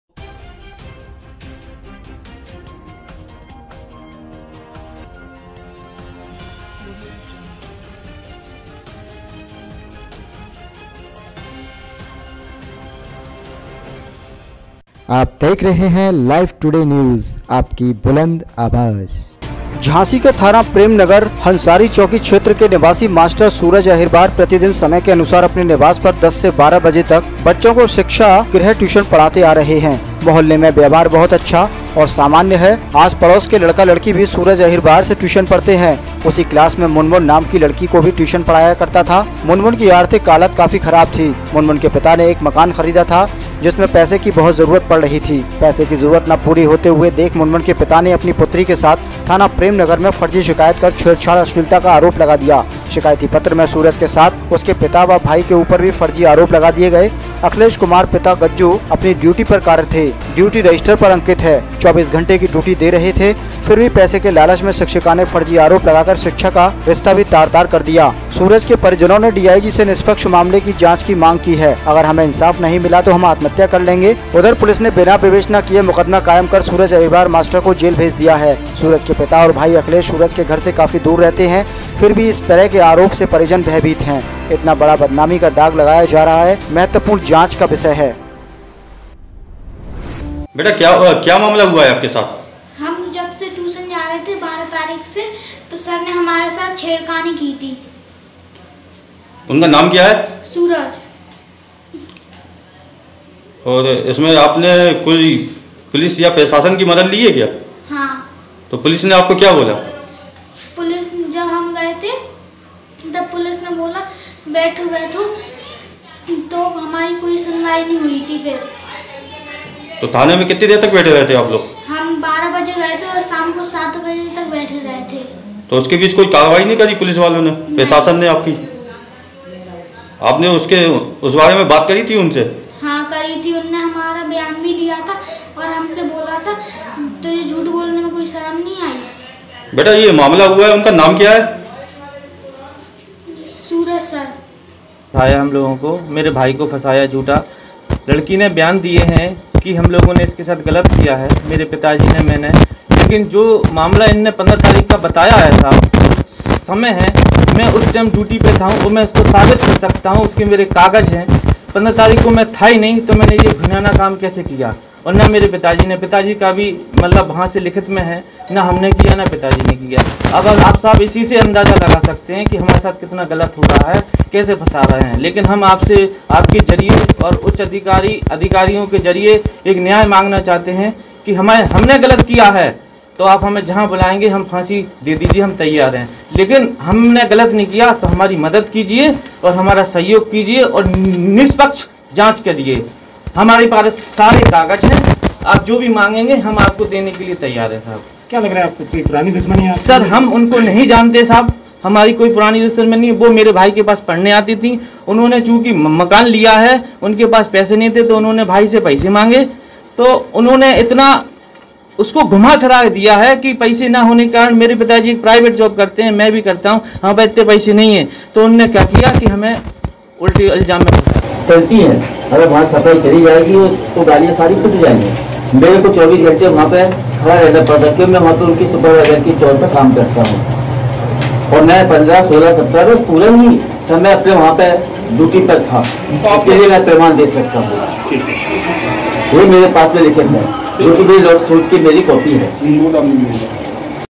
परिजनों ने लगाया आरोप, अगर हमें इंसाफ नहीं मिला तो कर लेंगे आत्महत्या
झांसी थाना प्रेम नगर  हंसारी चौकी क्षेत्र के  निबासी  मास्टर सूरज अहिरवार प्रतिदिन समय के अनुसार अपने निवास पर 10 बजे से 12: बजे तक बच्चों को शिक्षा गृह ट्यूशन पढ़ाता आ रहे है  मोहल्ले में व्यवहार  बहुत अच्छा और सामान है आस पास पड़ोस के  लड़का लड़की भी सूरज अहिरवार से ट्यूशन पढ़ते हैं उसी क्लास में मुनमुन नाम की लड़की को भी ट्यूशन पढ़ाया करता है मुनमुन की आर्थिक हालात काफी खराब थी  मुनमुन के पिता ने एक मकान खरीदा था जिसमें पैसे की बहुत जरूरत पड़ रही थी पैसे की जरूरत ना पूरी होते देख मुनमुन के पिता ने अपनी पुत्री के साथ थाना प्रेम नगर में फर्जी शिकायत कर छेड़छाड़ अश्लीलता का आरोप लगा दिया शिकायती पत्र में सूरज के साथ  उसके पिता ब भाई के  ऊपर भी फर्जी आरोप लगा दिया अखिलेश कुमार पिता गज्जू  अपनी ड्यूटी पर कार्य थे  ड्यूटी रजिस्टर पर अंकित है 24 घंटे की ड्यूटी दे रहे थे फिर भी पैसे के  लालच में शिक्षिका ने  फर्जी आरोप लगाकर शिक्षा का रिश्ता भी  तार-तार कर दिया सूरज के परिजनों ने डीआईजी महोदय से निष्पक्ष जांच की मांग की है अगर हमें इंसाफ नहीं मिला तो हम आत्महत्या कर लेंगे उधर पुलिस ने बिना विवेचना किए मुकदमा कायम कर सूरज अहिरवार मास्टर को जेल भेज दिया है सूरज के पिता और भाई अखिलेश सूरज के घर से काफी दूर रहते हैं फिर भी इस तरह के आरोप से परिजन भयभीत है इतना बड़ा बदनामी का दाग लगाया जा रहा है महत्वपूर्ण जांच का विषय है